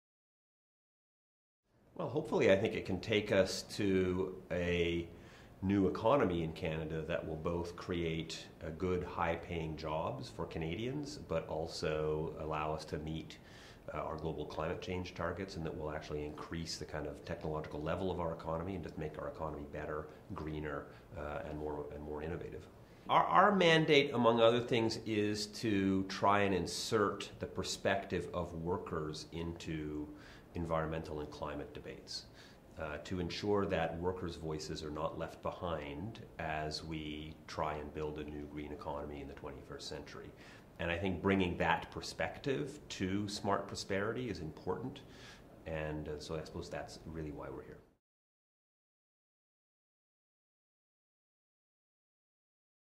Mark Rowlinson - Smart Prosperity
Mark Rowlinson is the President of Blue Green Canada, an alliance of Canadian labour unions, environmental and civil society organizations that advocates for working people and the environment by promoting solutions to environmental issues that have positive employment and economic impacts.

Rowlinson is also the Executive Assistant to the Canadian National Director of the United Steelworkers, a senior administrative and policy position within the Union.

Before joining the Steelworkers, Mark completed degrees at McGill University in Montreal, the University of Geneva Switzerland, and Osgoode Hall Law School in Toronto, and was called to the Bar of Ontario in 1994. From 1994 to 2011, Mark worked as Counsel to the Steelworkers in Toronto, where he represented the Union before courts and tribunals across Canada.